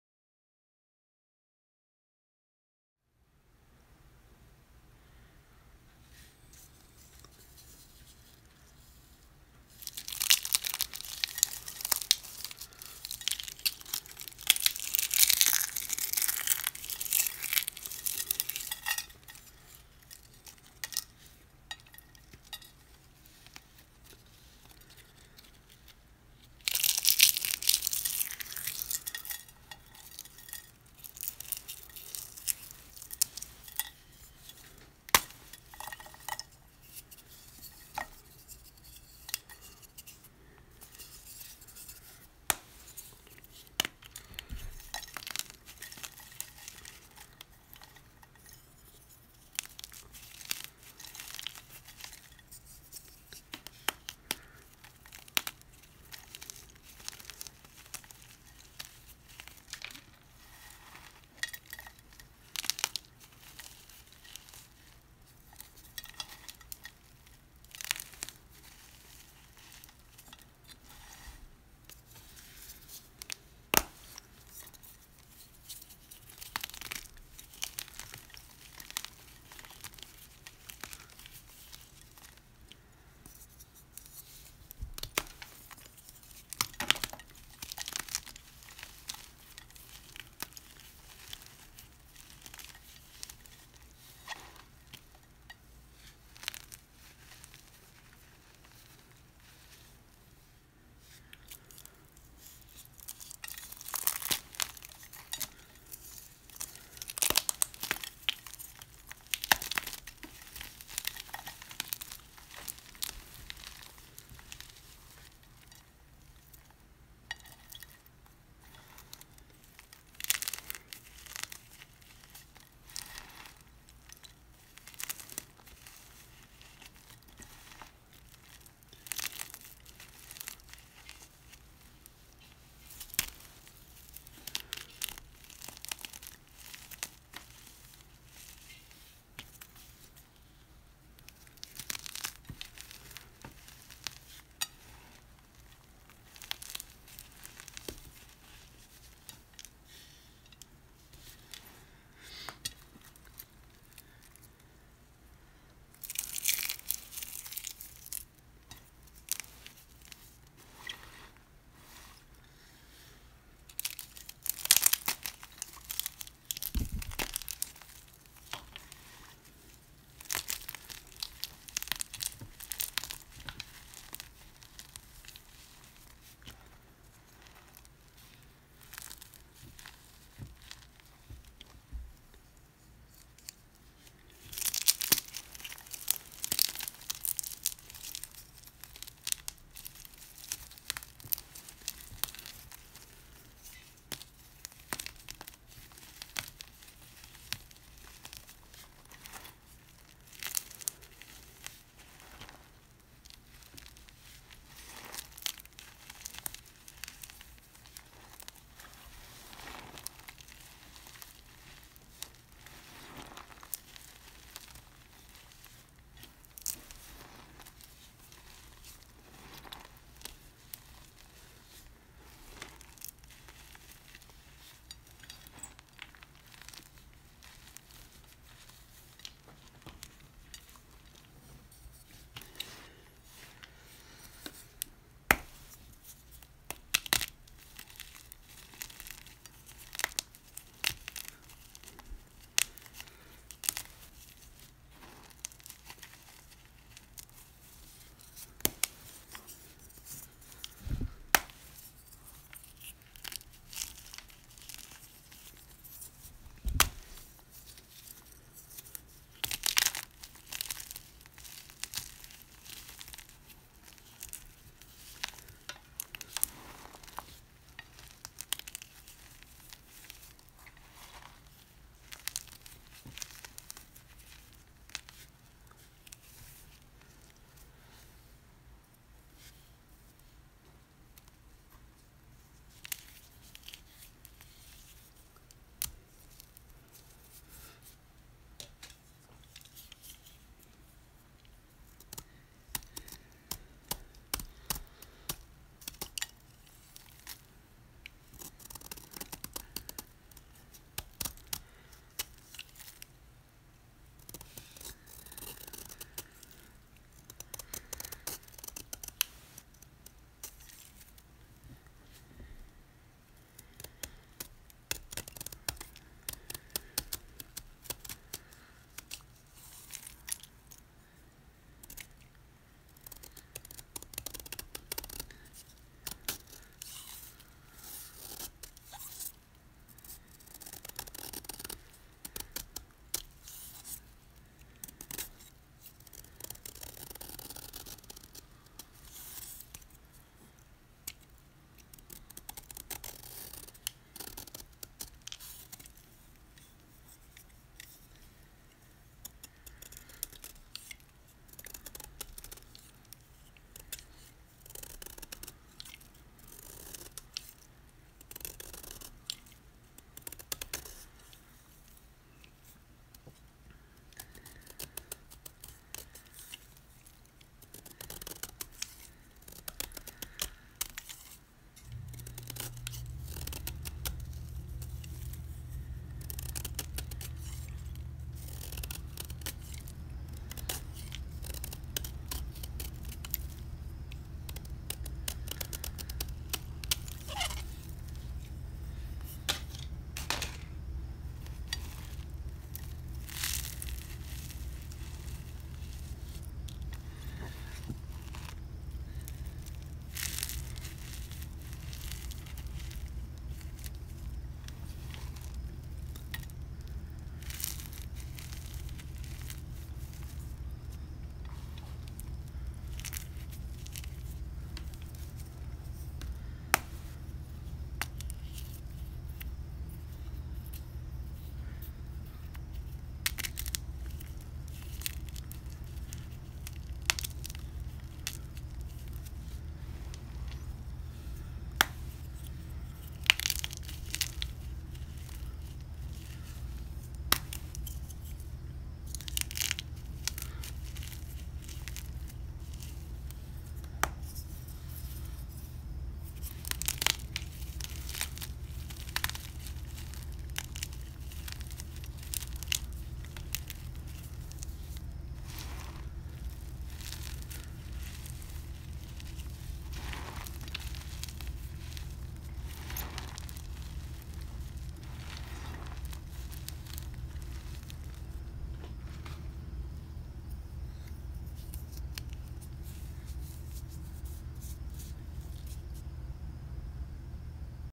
Giant Chalk Block Crush | Crunchy ASMR
My first gym chalk video! What do you guys think? Comment if you want more and other things you’d like to see!

Main: ASMRjunky
Xtras: ASMRjunk